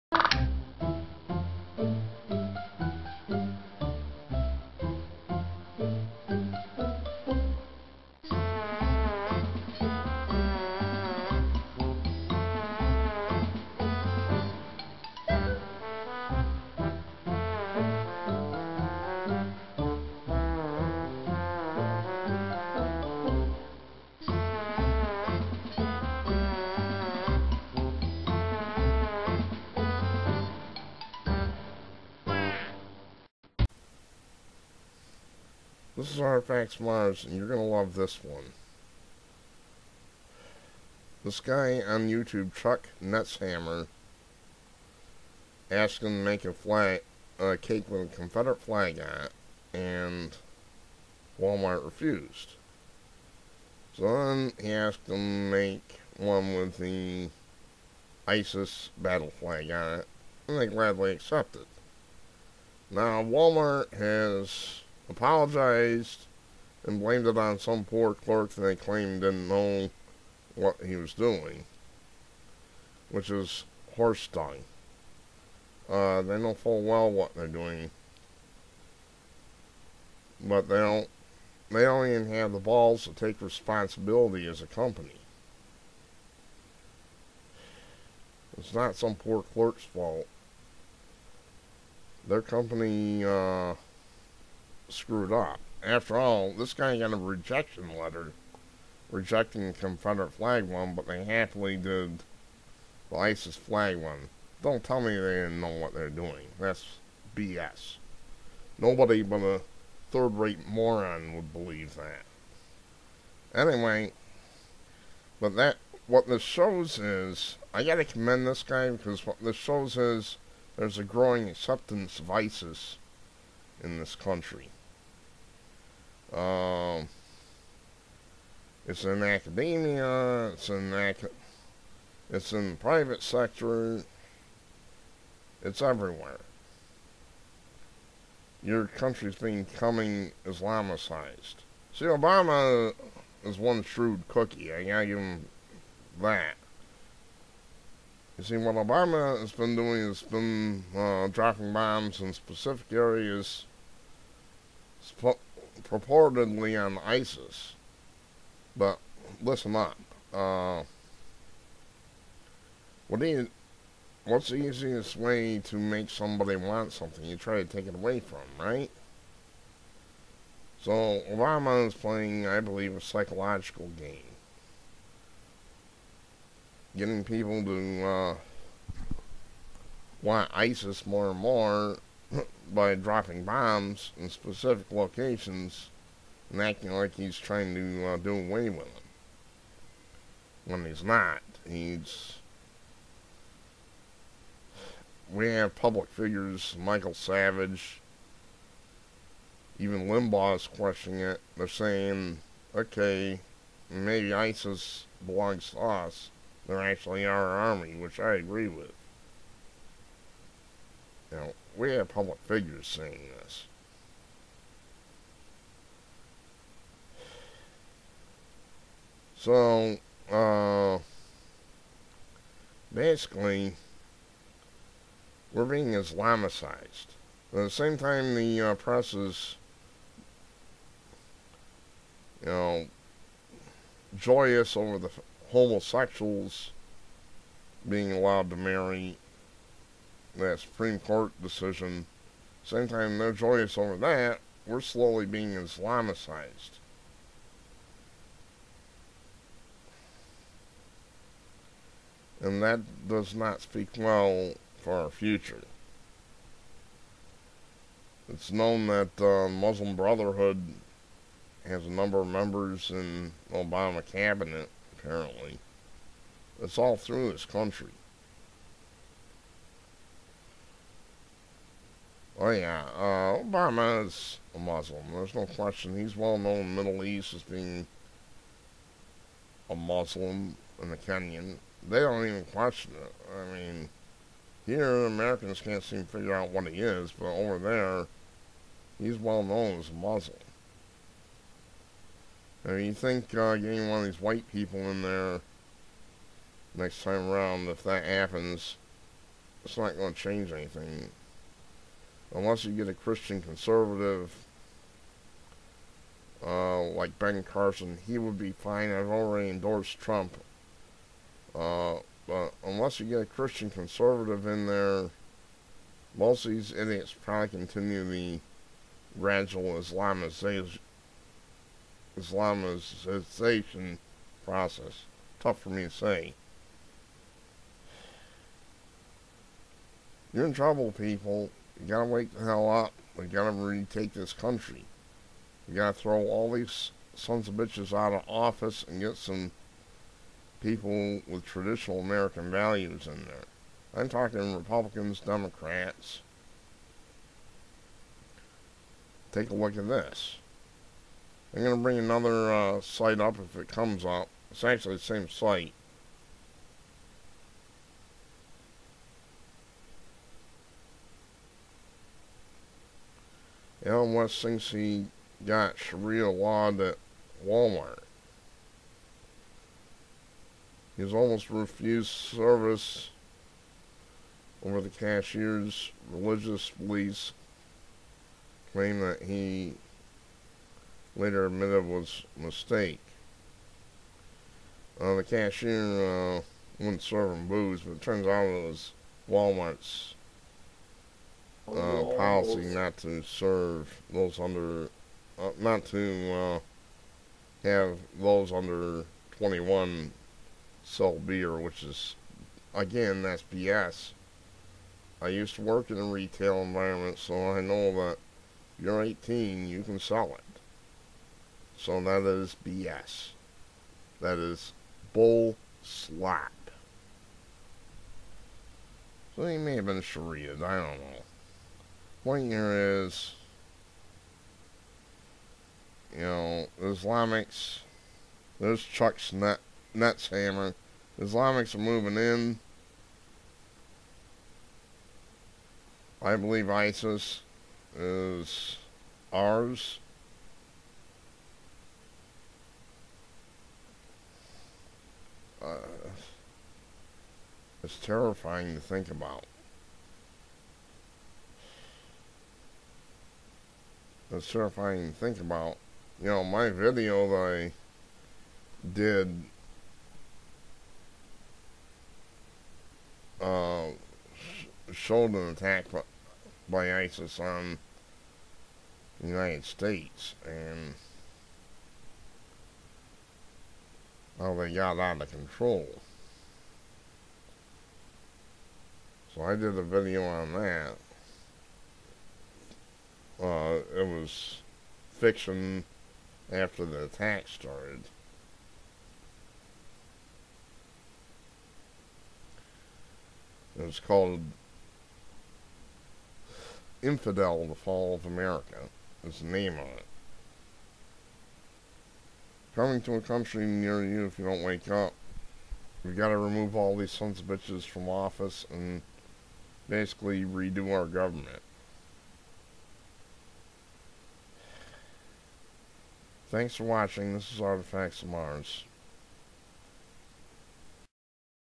Walmart bakes an ISIS cake, but not a Confederate one
A You-tuber, after having his Confederate flag cake rejected by Wal-Mart, requested one with the ISIS flag on it. Wal-Mart gladly complied. After public anger forced them to apologize, they had the nerve to blame the ISIS flag cake on a defenseless clerk. Liberalism is a very dangerous mental disorder. Acceptance of ISIS is growing rapidly in academia and even in the public sector.
  Many public figures are saying, and I implied in my You-tube movie "Infidel! The fall of America", that ISIS is under US control. Obama is a crafty slimeball who uses fake airstrikes to invoke reverse psychology, drumming up support for ISIS. They are supported by academia, and yes, the private sector too as this illustrates.